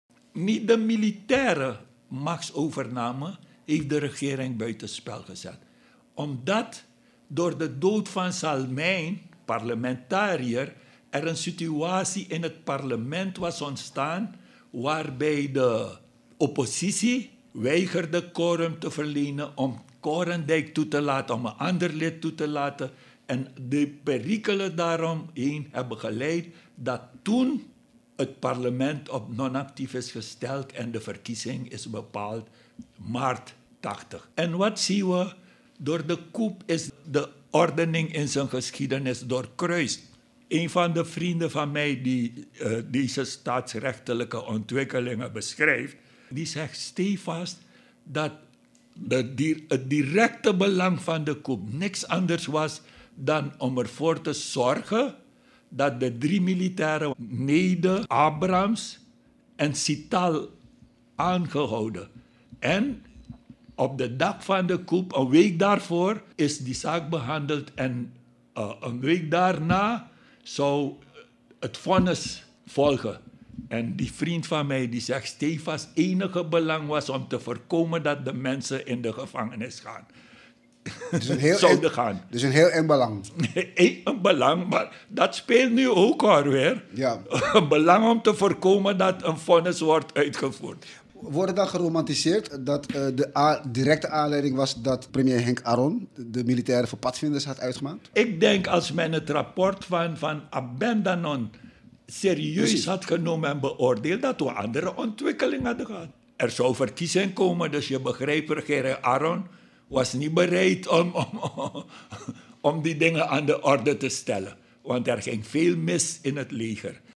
‘Persoonlijke enge belangen waren directe aanleiding voor militaire coup’ - ABC Online Nieuws
ABC Online Nieuws - 25 februari 2022

‘Persoonlijke enge belangen waren directe aanleiding voor militaire coup’

Achter de militaire coup van 25 februari 1980, vandaag 42 jaar geleden, ging geen grote ideologie schuil. Laat staan dat het te maken had met een revolutie. Het ging om enge belangen op persoonlijk niveau.

Dit zegt oud-advocaat-generaal Armand van der San. Hij was in 1980 agent van het politiekorps. Hoewel met de coup de Surinaamse democratie gewelddadig terzijde werd geschoven, had het buiten spel zetten van het landsbestuur feitelijk een andere oorzaak. Zo stelt van der San.